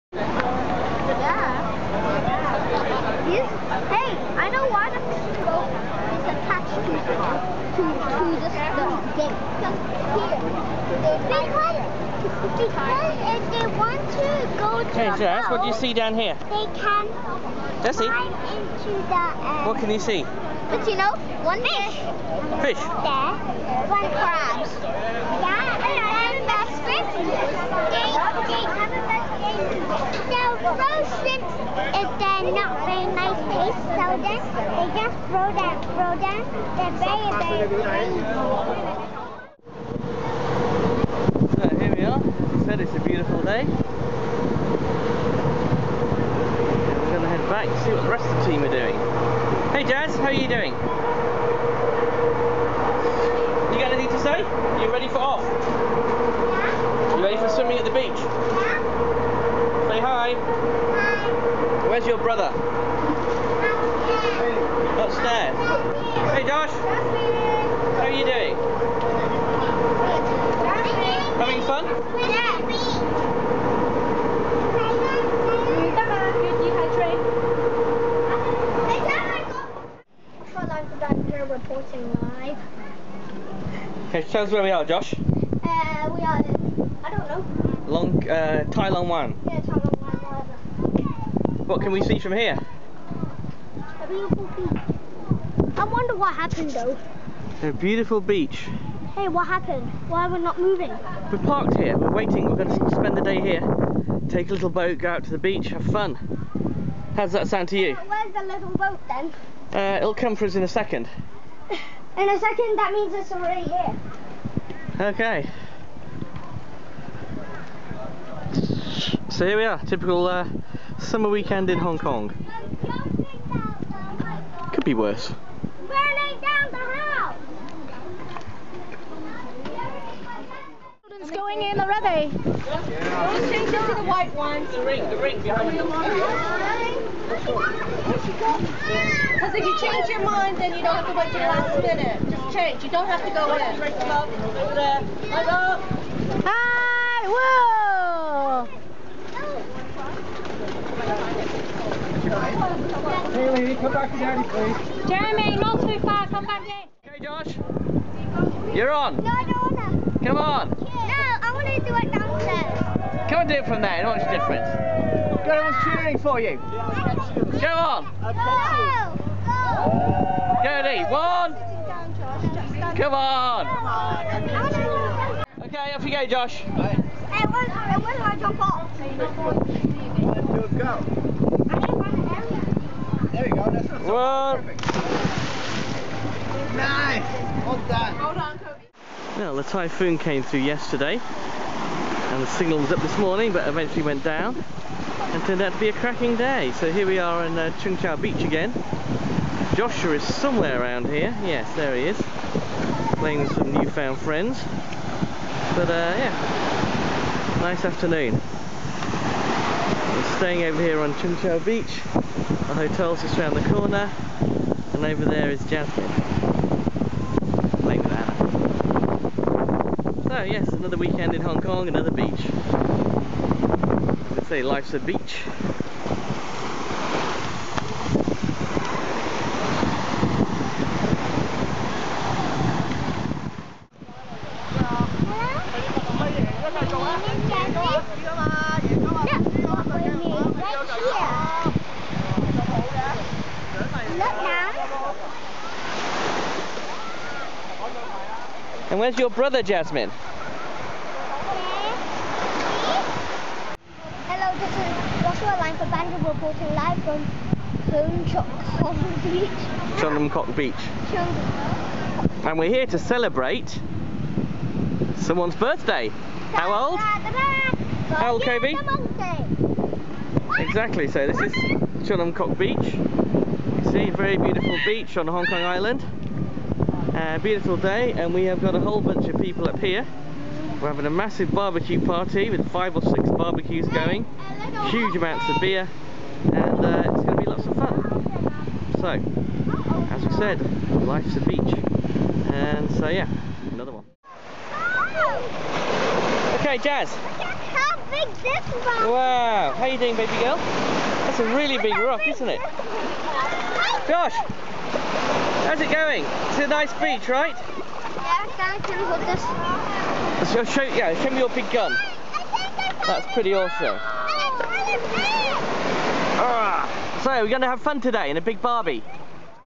116 Life's A Beach (2, 22, 29Jun13)
The glorious summer weather arrives. With temperatures in the mid-30s where better to spend the weekends than on the beach. A sample of some of Hong Kong's better beaches. Junk trips, BBQs, and fun in the sun - summer in Hong Kong!